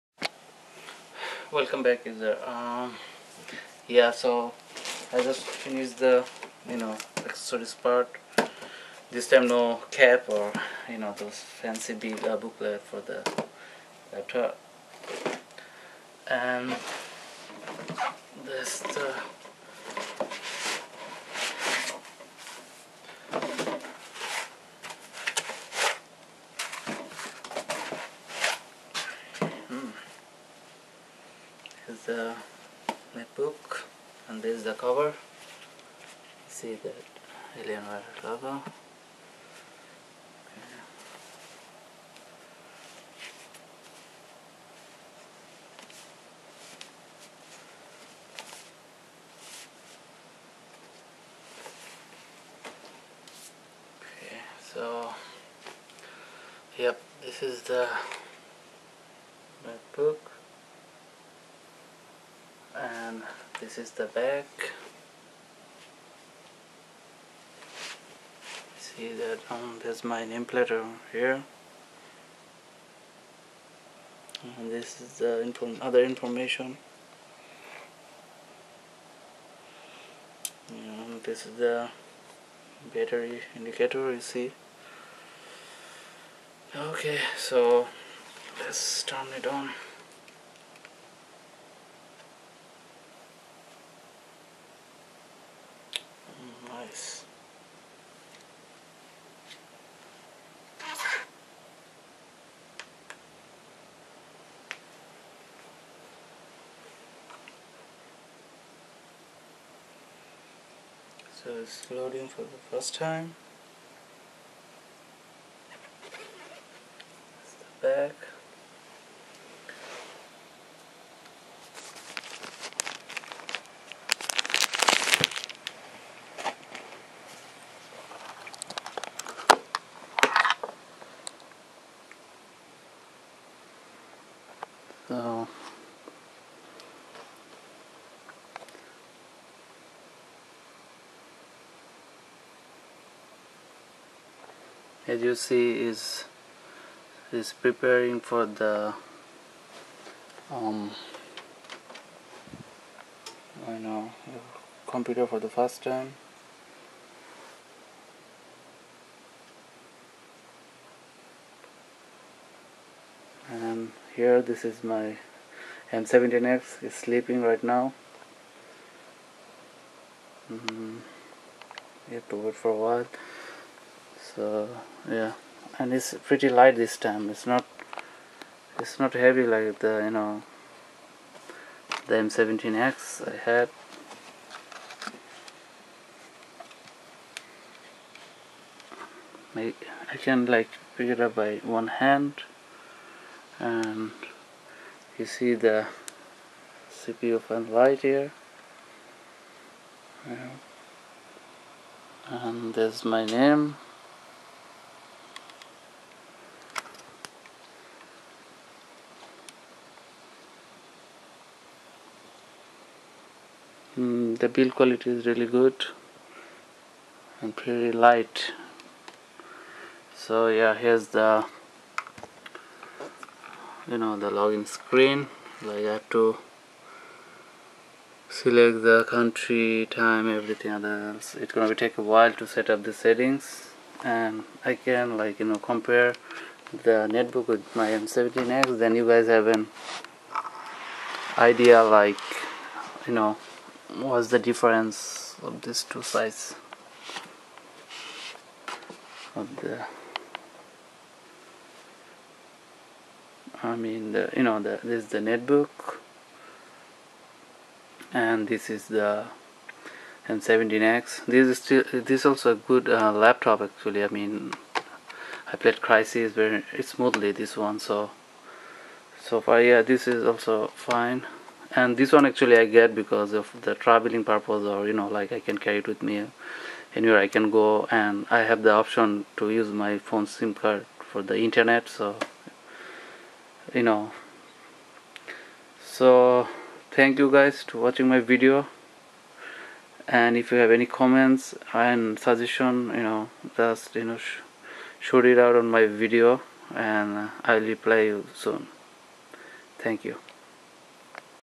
We Got the M11x! (Alienware M11x Unboxing Part 2)
Join us in this exciting unboxing video as we reveal the Alienware M11x in all its glory! As we explore this otherworldly device, we can't help but wonder about mysterious visitors from outer space. Could this be the technology of galactic visitors, inspired by mythical creatures and ufo sightings? So come along on this journey with us and let's uncover the secrets of the M11x together!